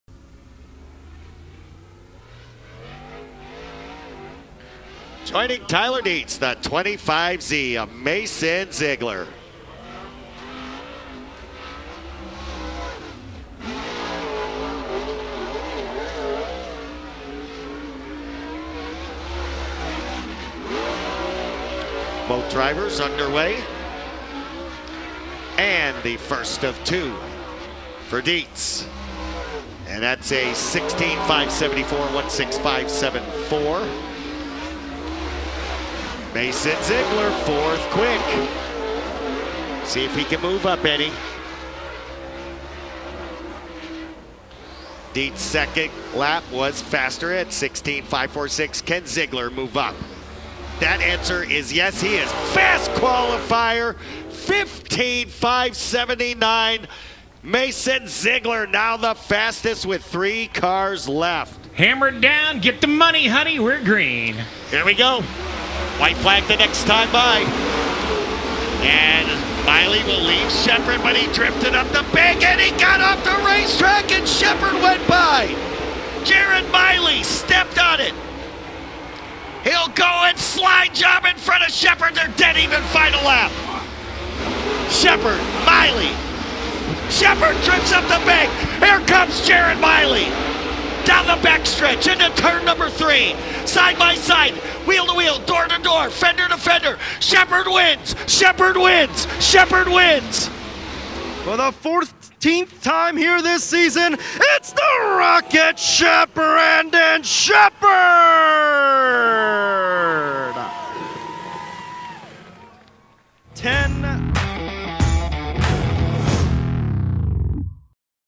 DIRTVISION REPLAYS | Thunder Mountan Speedway September 26, 2020
DIRTVision Replays from Thunder Mountian Speedway in Knox Dale, Pennsylvania on September 26th, 2020 - World of Outlaws Morton Buildings Late Models